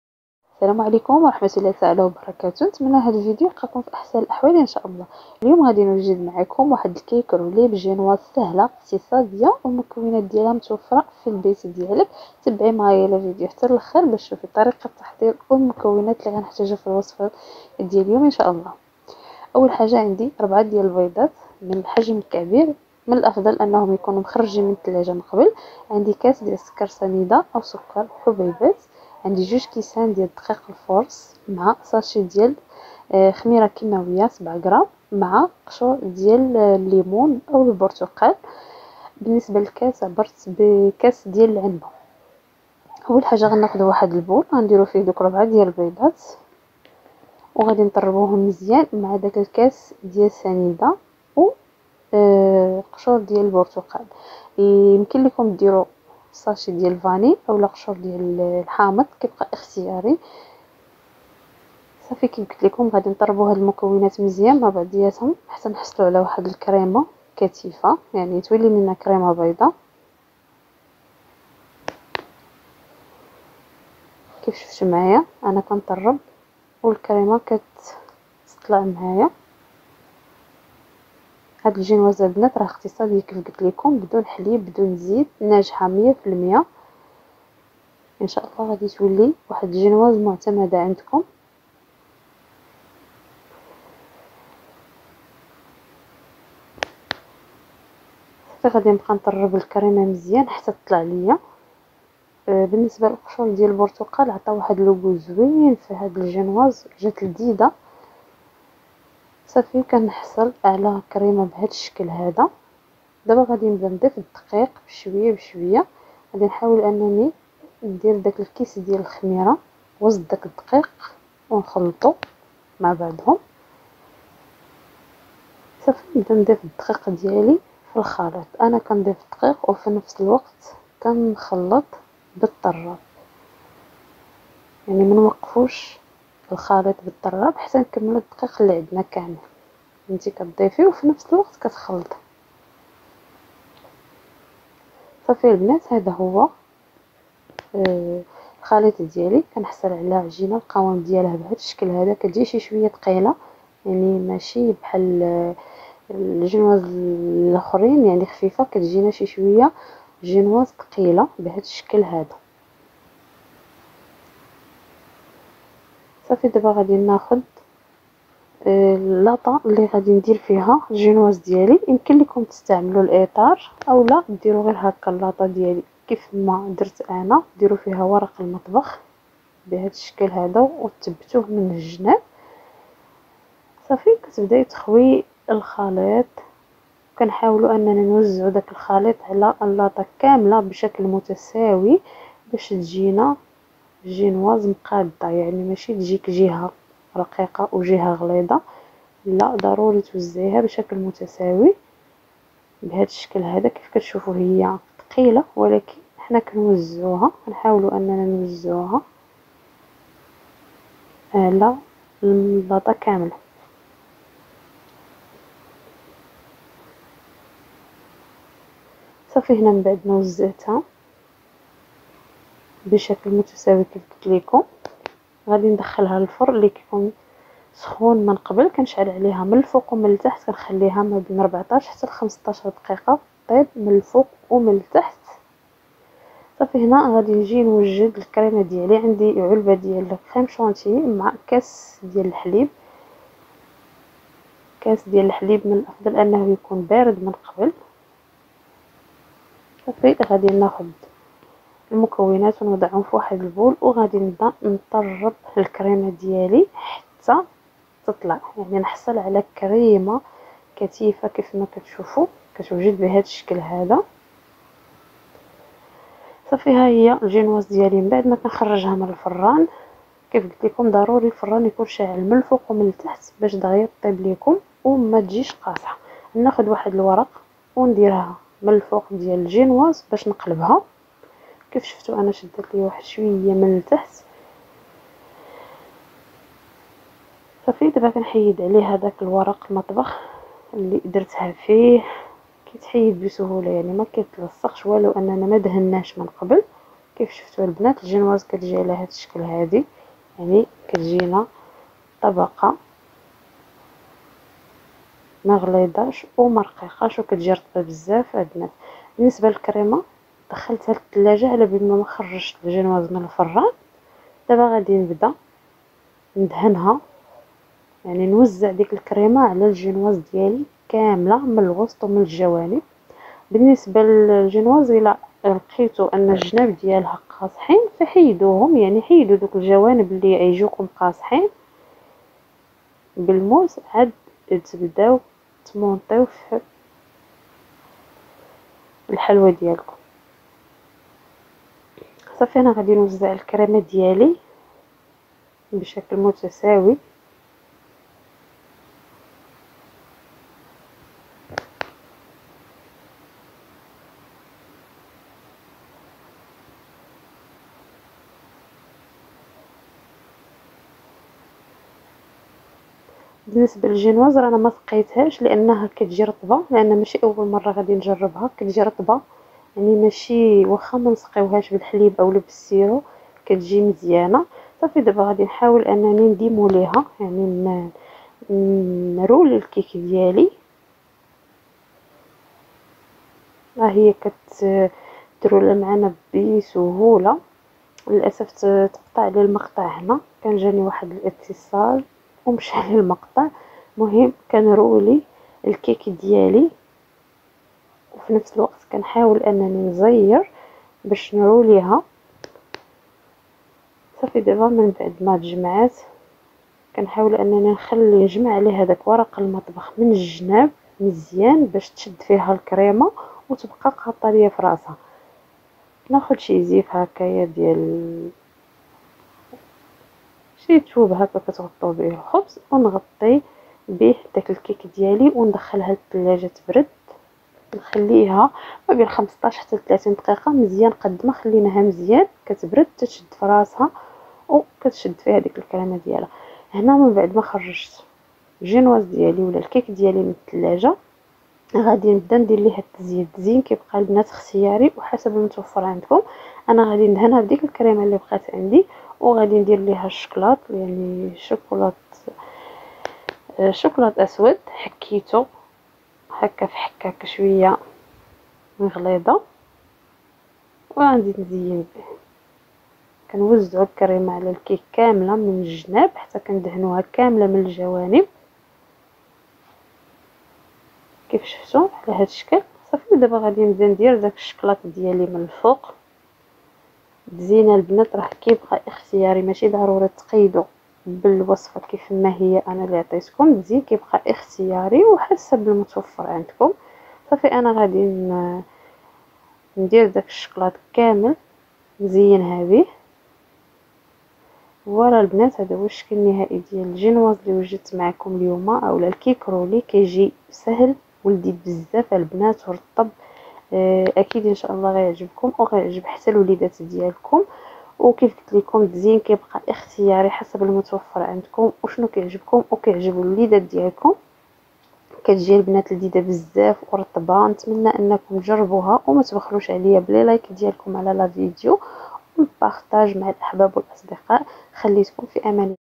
كيك رولي بجنواز اقتصادية بدون زيت او حليب وصفة سهلة للمبتدئات بمكونات بسيطة فرحي وليداتك
قناة asmaa oum imrane كتقدم ليكم كيك رولي سهل واقتصادي نتمنى يعجبكم ودعموني بلايك ولاشتراك على لقناة
    المكونات:كاس سكر سنيدة
    ٢كوؤوس دقيق لفورص
    خمارة ١ (بيكن باودر)
    4 بيضات بحجم كبير
     قشور ليمون (برتقال)
  لعبار بكاس لعنبة